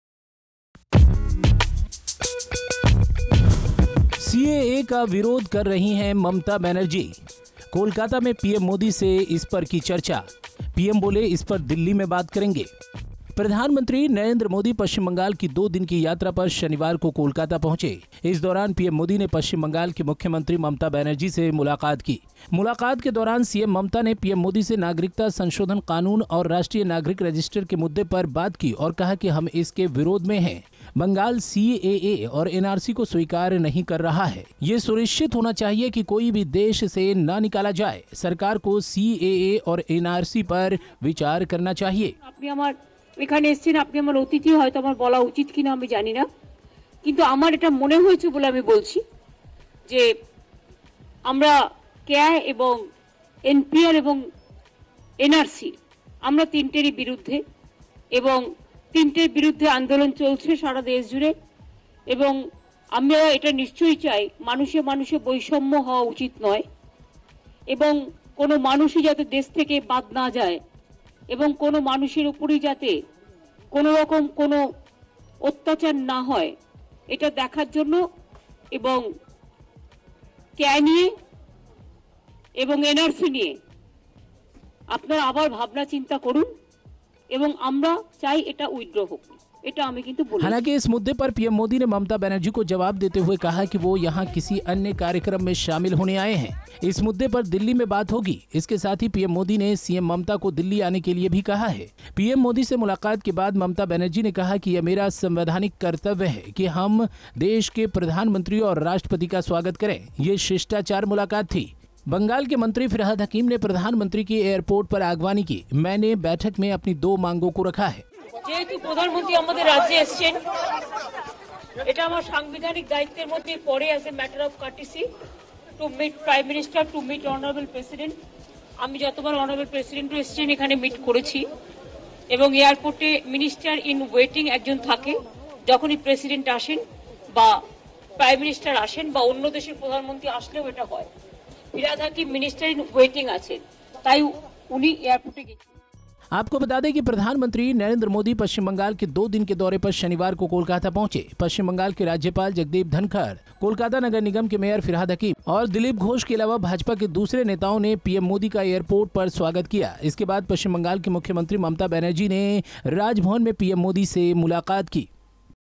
प्रधानमंत्री नरेंद्र मोदी दो दिवसीय दौरे पर कोलकाता पहुंच गये
प्रधानमंत्री नरेंद्र मोदी दो दिवसीय दौरे पर कोलकाता पहुंच गये हैैं| जहां एयरपोर्ट पर राज्‍य सराकर के मंत्री फिरहाद हकीम, बंगाल भाजपा अध्यक्ष दिलीप घोष, राज्यपाल ने उनका स्वागत किया|

Download the taazatv app for latest updates: android.taazatv.com | ios.taazatv.com